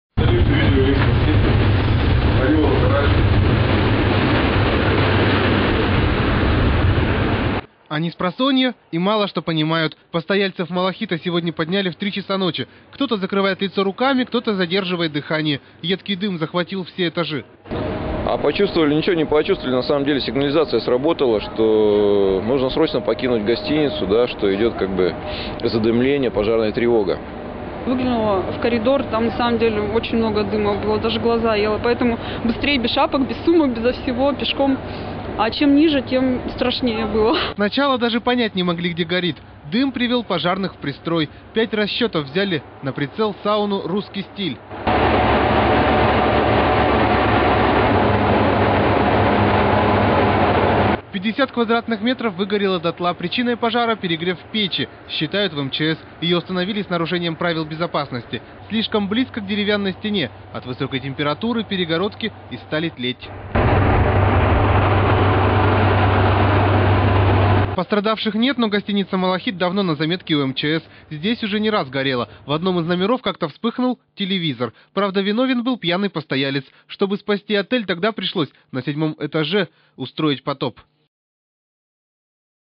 ! POG MALAXIT OUT
В гостинице на Труда была объявлена срочная эвакуация.